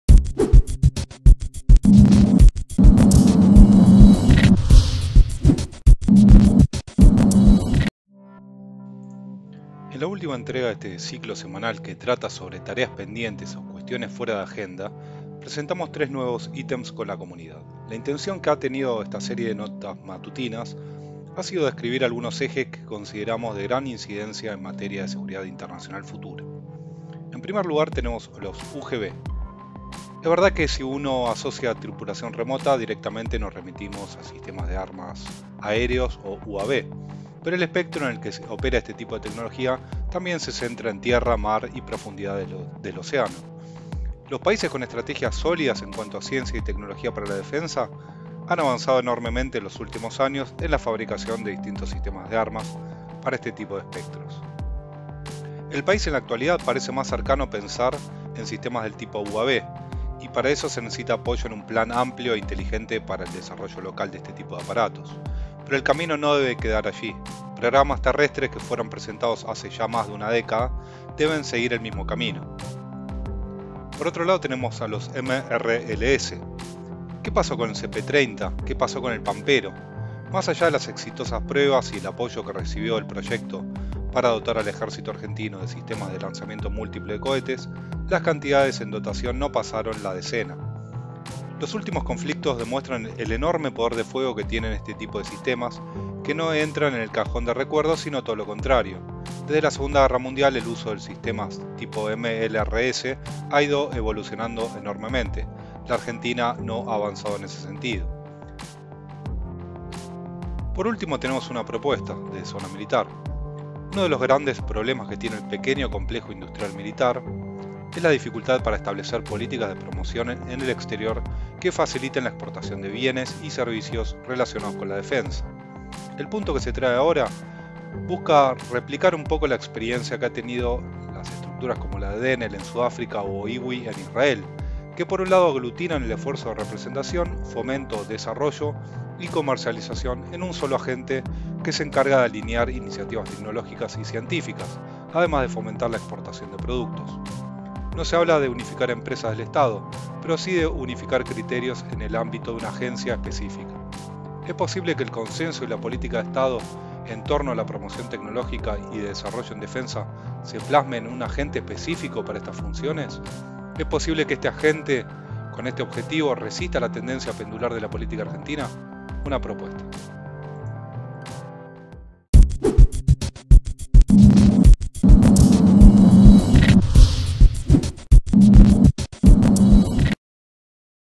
Tareas pendientes para las Fuerzas Armadas: UGV, MRLS, ARGDEF
En la ultima entrega de este ciclo semanal que trata sobre tareas pendientes o cuestiones fuera de agenda, presentamos tres nuevos items con la comunidad. La intención que han tenido esta serie de notas matutinas ha sido describir algunos ejes que consideramos de gran incidencia en materia de seguridad internacional futura.

Si bien a lo largo de los distintos puntos desarrollados en la semana hemos encontrado algunos que pueden ser inabordables dadas las enormes dificultades que se presentan en la actualidad, comenzar a pensar esos temas, ponerlos en la agenda y planificar de cara a contextos mas favorables puede acelerar algunos tiempos futuros.